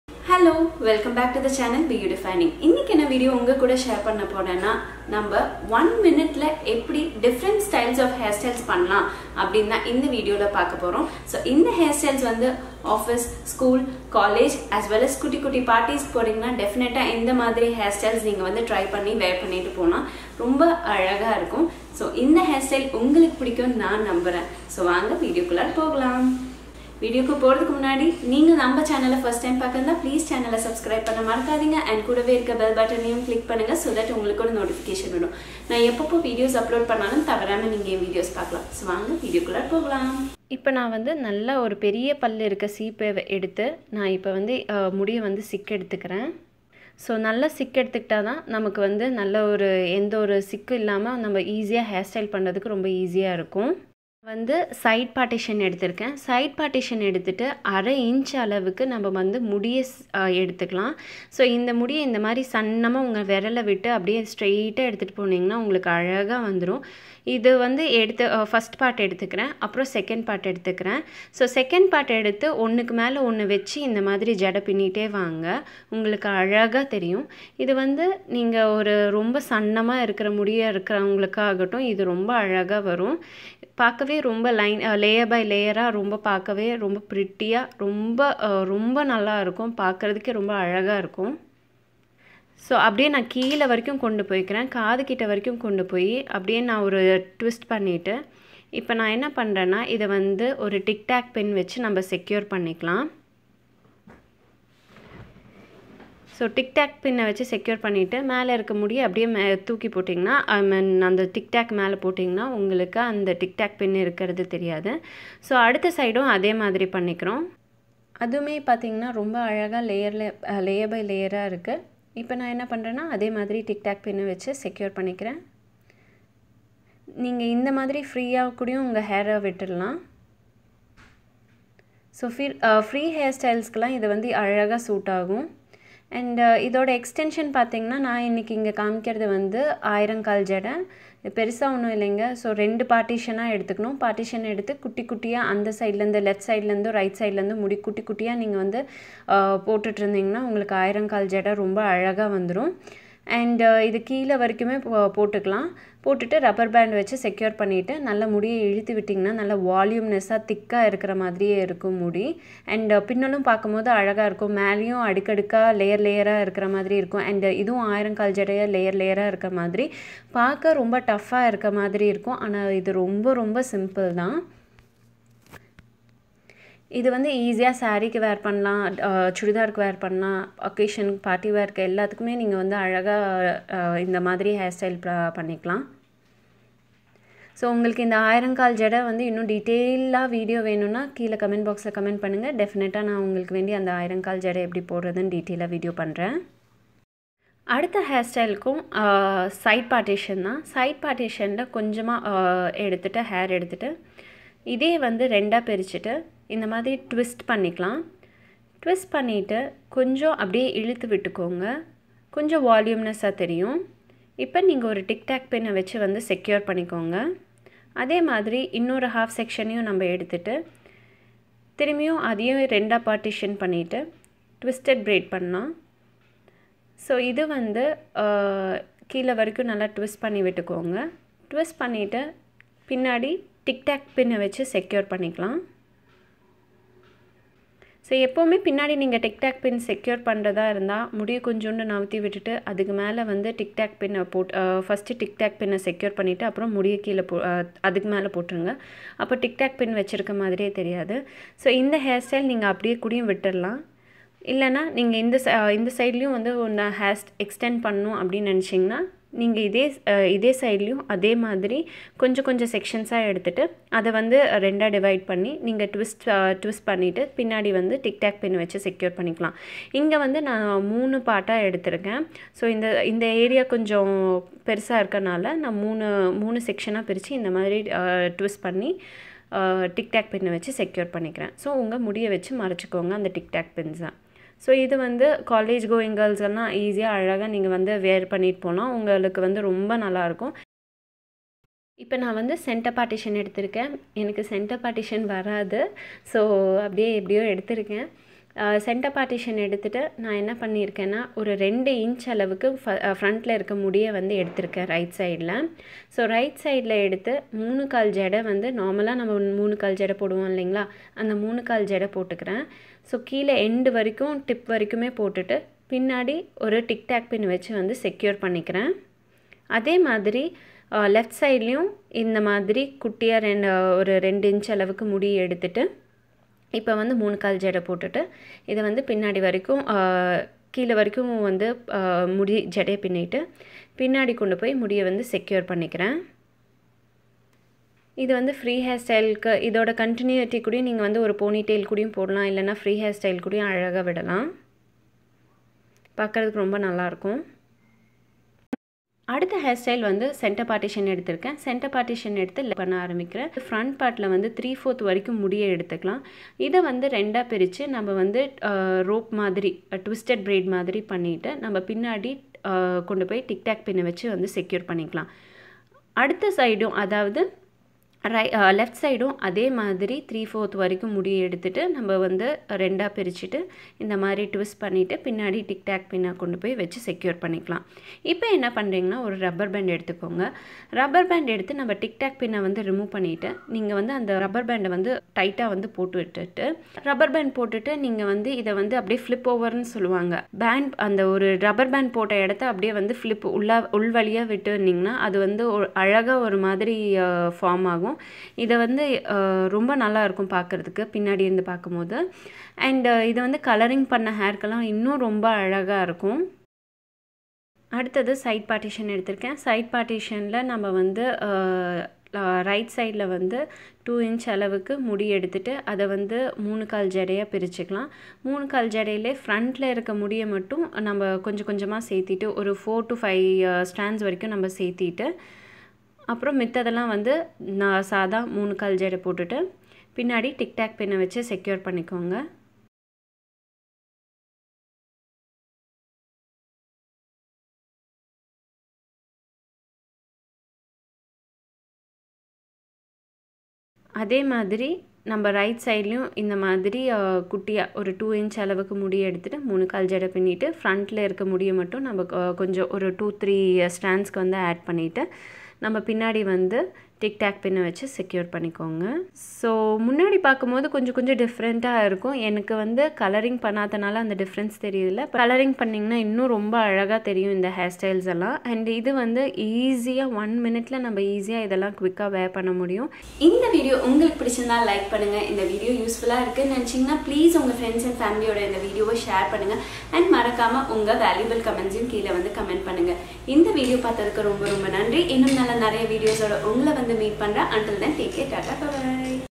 1 minute simple & easy do hairstyles for School, College & Office || Tamilyoutuber || beyoudefining
Dear Beauties,

In this video, I am going to show you how to do 1 minute simple & easy to do hairstyles for School, College & Office goers.
Hope this video will keep you in style and fashion, to avoid the routine daily boring hairstyle.